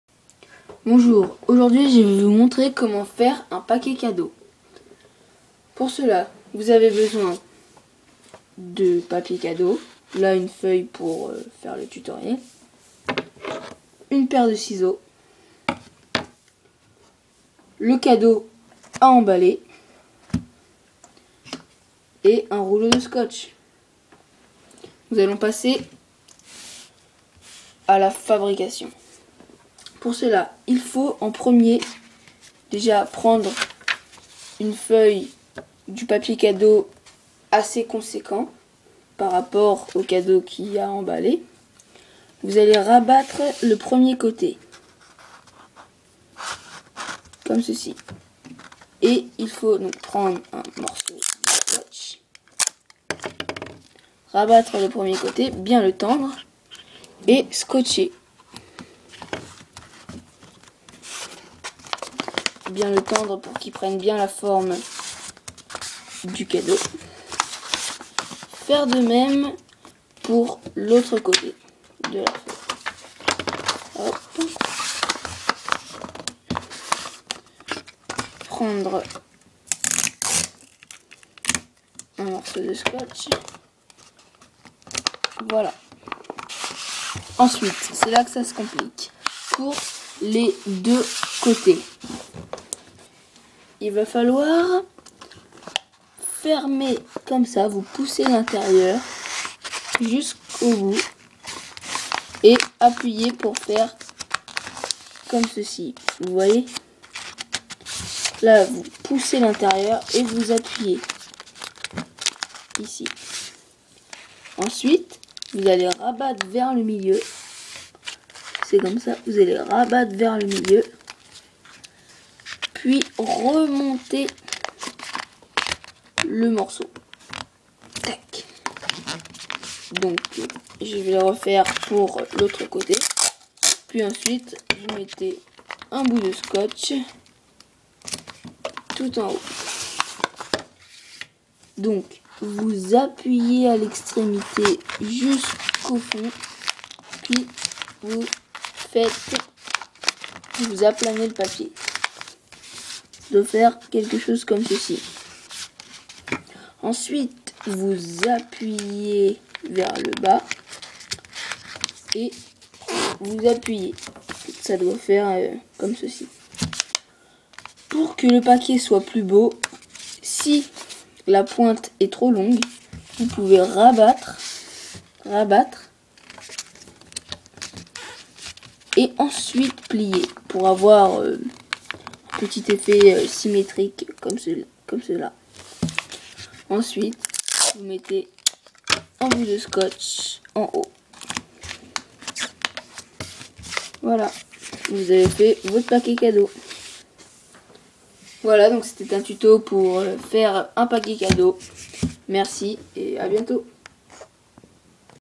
Emballer un cadeau
Ce tuto vous montrera comment emballer un cadeau de façon à ce qu'il soit vraiment bien réussi.